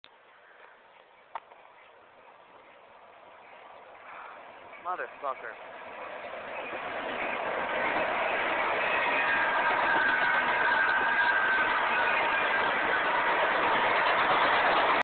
Lisle: Good consist
BN SD40-2 and a BNSF B40-8 lead a bare table train west.